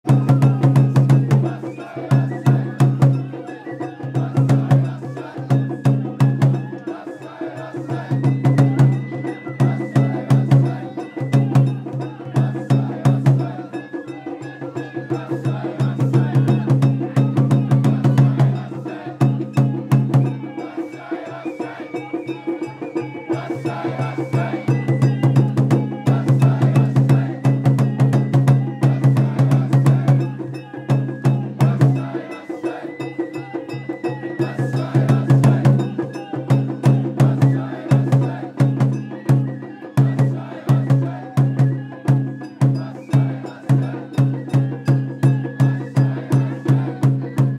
本町の太鼓台 七町合同曳き回し 二本松のちょうちん祭り2018 本祭り 00001
本町の太鼓台
七町合同曳き回し
二本松のちょうちん祭り2018　本祭り
2018年10月5日